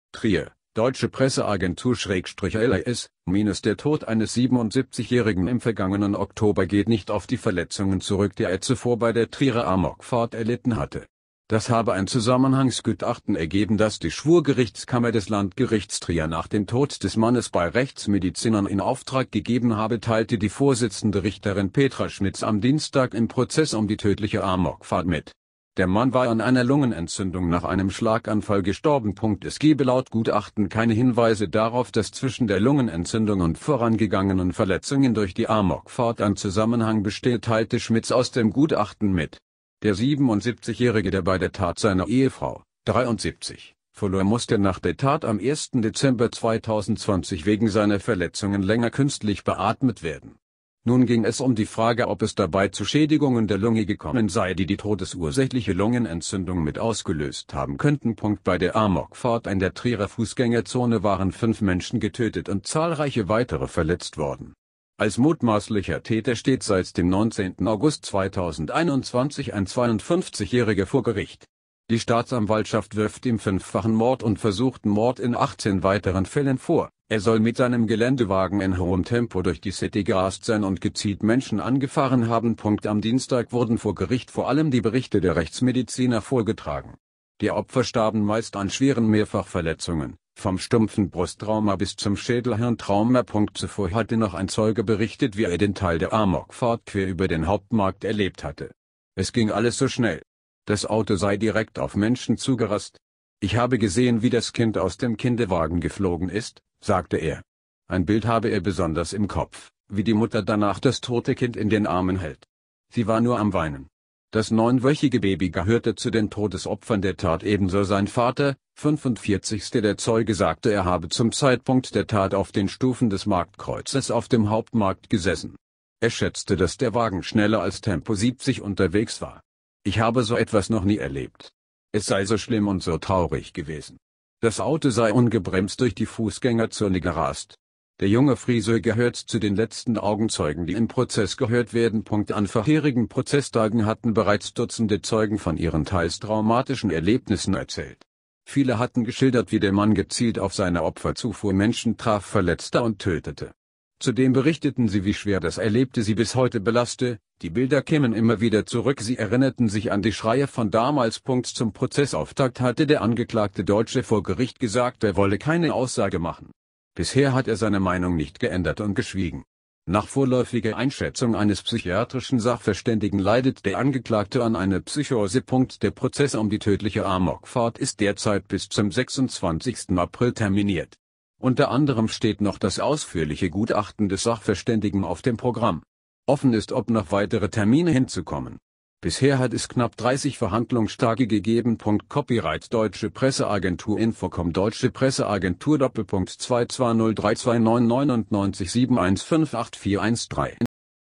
Letzte Augenzeugen und Berichte im Amokfahrt-Prozess
Trier (dpa/lrs) - Der Tod eines 77-Jährigen im vergangenen Oktober geht nicht auf die Verletzungen zurück- die er zuvor bei der Trierer Amokfahrt erlitten hatte. Das habe ein "Zusammenhangsgutachten" ergeben- das die Schwurgerichtskammer des Landgerichts Trier nach dem Tod des Mannes bei Rechtsmedizinern in Auftrag gegeben habe- teilte die Vorsitzende Richterin Petra Schmitz am Dienstag im Prozess um die tödliche Amokfahrt mit. Der Mann war an einer Lungenentzündung nach einem Schlaganfall gestorben.Es gebe laut Gutachten keine Hinweise darauf- dass zwischen der Lungenentzündung und vorangegangenen Verletzungen durch die Amokfahrt ein Zusammenhang bestehe- teilte Schmitz aus dem Gutachten mit. Der 77-Jährige- der bei der Tat seine Ehefrau (73) verlor- musste nach der Tat am 1. Dezember 2020 wegen seiner Verletzungen länger künstlich beatmet werden. Nun ging es um die Frage- ob es dabei zu Schädigungen der Lunge gekommen sei- die die todesursächliche Lungenentzündung mit ausgelöst haben könnten.Bei der Amokfahrt in der Trierer Fußgängerzone waren fünf Menschen getötet und zahlreiche weitere verletzt worden. Als mutmaßlicher Täter steht seit dem 19. August 2021 ein 52-Jähriger vor Gericht. Die Staatsanwaltschaft wirft ihm fünffachen Mord und versuchten Mord in 18 weiteren Fällen vor: Er soll mit seinem Geländewagen in hohem Tempo durch die City gerast sein und gezielt Menschen angefahren haben.Am Dienstag wurden vor Gericht vor allem die Berichte der Rechtsmediziner vorgetragen. Die Opfer starben meist an schweren Mehrfachverletzungen - vom stumpfen Brusttrauma bis zum Schädel-Hirn-Trauma.Zuvor hatte noch ein Zeuge berichtet- wie er den Teil der Amokfahrt quer über den Hauptmarkt erlebt hatte. "Es ging alles so schnell." Das Auto sei direkt auf Menschen zugerast. "Ich habe gesehen- wie das Kind aus dem Kinderwagen geflogen ist"- sagte er. Ein Bild habe er besonders im Kopf: "Wie die Mutter danach das tote Kind in den Armen hält. Sie war nur am Weinen." Das neunwöchige Baby gehörte zu den Todesopfern der Tat- ebenso sein Vater (45).Der Zeuge sagte- er habe zum Zeitpunkt der Tat auf den Stufen des Marktkreuzes auf dem Hauptmarkt gesessen. Er schätzte- dass der Wagen schneller als Tempo 70 unterwegs war. "Ich habe so etwas noch nie erlebt." Es sei "so schlimm und so traurig" gewesen. Das Auto sei ungebremst durch die Fußgängerzone gerast. Der junge Friseur gehört zu den letzten Augenzeugen- die im Prozess gehört werden.An vorherigen Prozesstagen hatten bereits Dutzende Zeugen von ihren teils traumatischen Erlebnissen erzählt. Viele hatten geschildert- wie der Mann gezielt auf seine Opfer zufuhr- Menschen traf- verletzte und tötete. Zudem berichteten sie- wie schwer das Erlebte sie bis heute belaste: Die Bilder kämen immer wieder zurück- sie erinnerten sich an die Schreie von damals.Zum Prozessauftakt hatte der angeklagte Deutsche vor Gericht gesagt- er wolle keine Aussage machen. Bisher hat er seine Meinung nicht geändert und geschwiegen. Nach vorläufiger Einschätzung eines psychiatrischen Sachverständigen leidet der Angeklagte an einer Psychose.Der Prozess um die tödliche Amokfahrt ist derzeit bis zum 26. April terminiert. Unter anderem steht noch das ausführliche Gutachten des Sachverständigen auf dem Programm. Offen ist- ob noch weitere Termine hinzukommen. Bisher hat es knapp 30 Verhandlungstage gegeben.© dpa-infocom- dpa:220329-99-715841/3